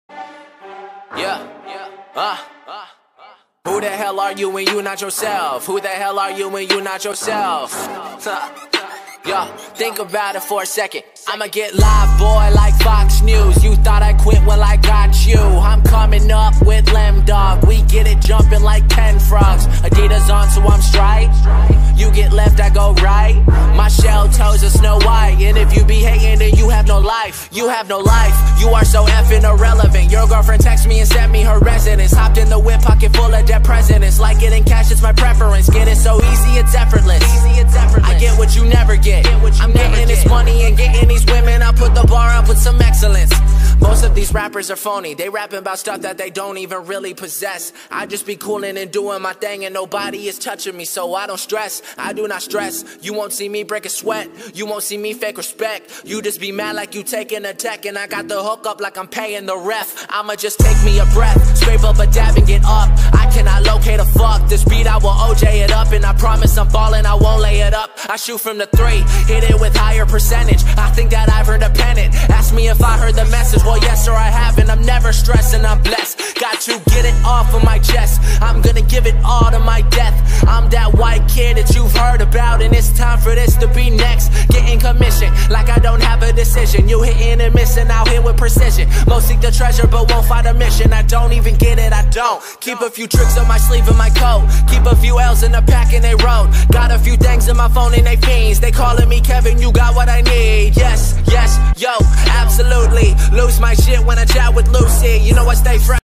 "STREET- WORKOUT" Dimitur Koprivlenski - Турнир гр.София "Mall of Serdika"
Hello Guys! this is one of my friends and this is his StreetWorkout set on the tournament in sofia city, he has very good skills ! Enjoy watching ;) !